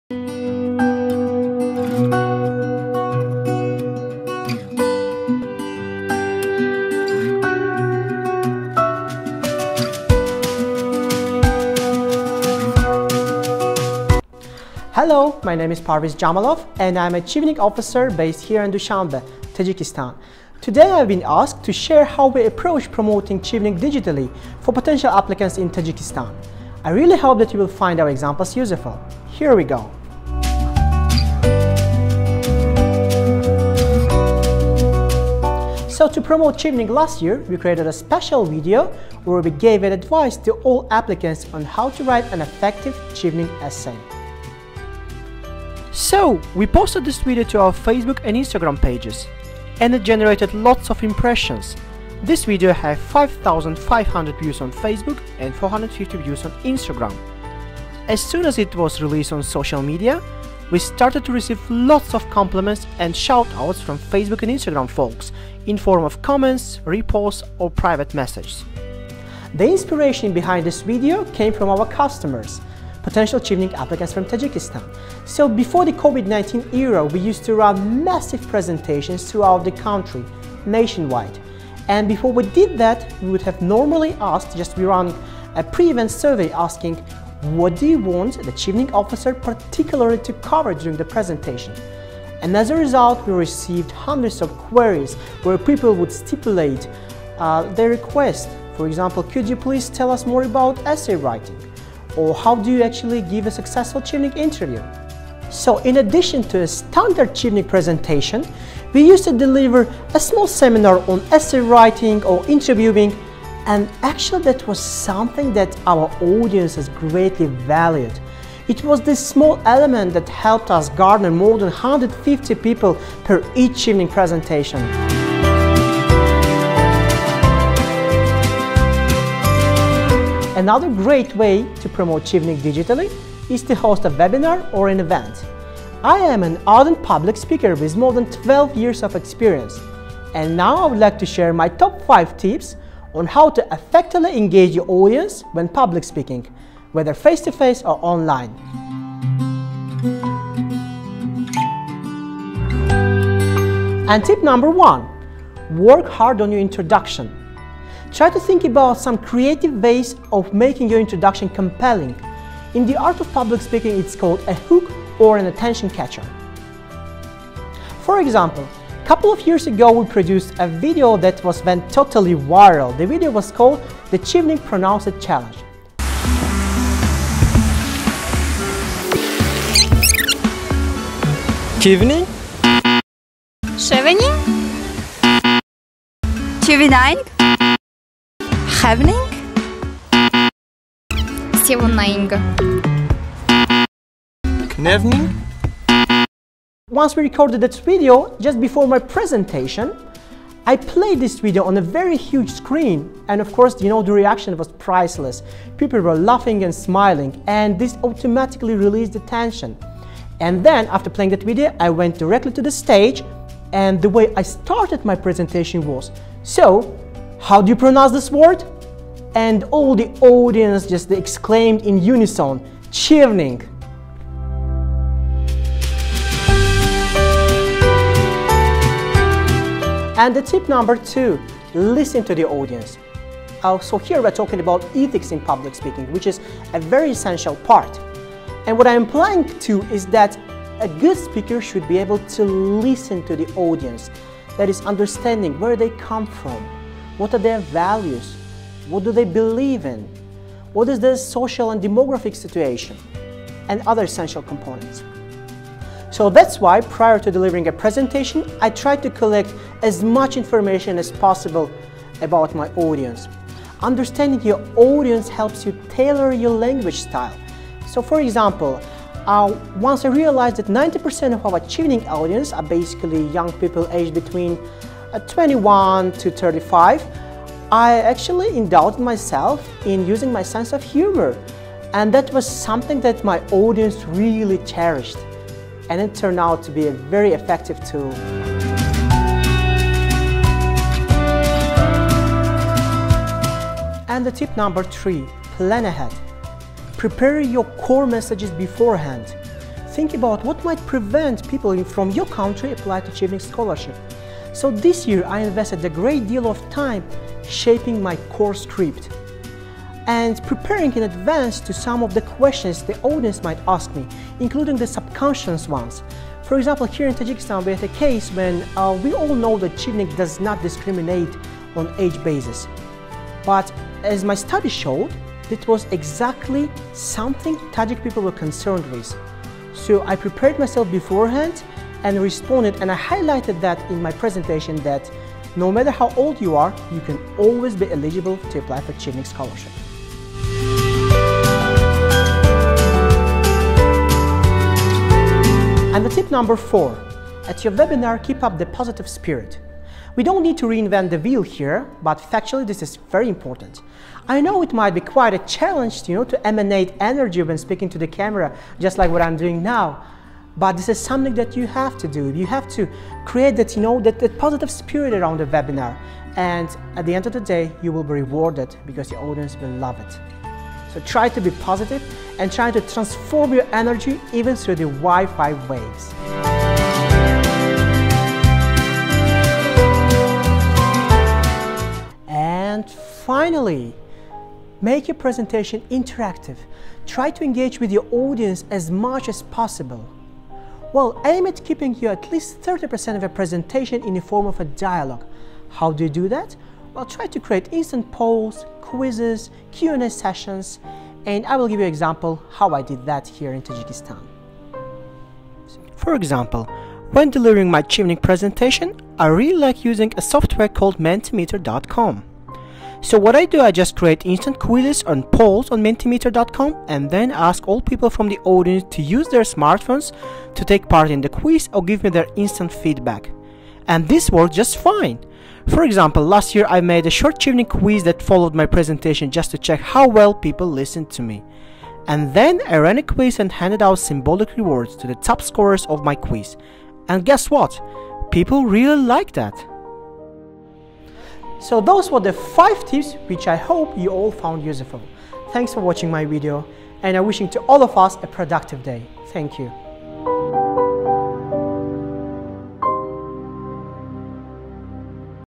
CheveningConference PJ Tajikistan
Looking for ideas to promote Chevening digitally? Post in Tajikistan talk about creating videos for social media and hosting an engaging online webinar!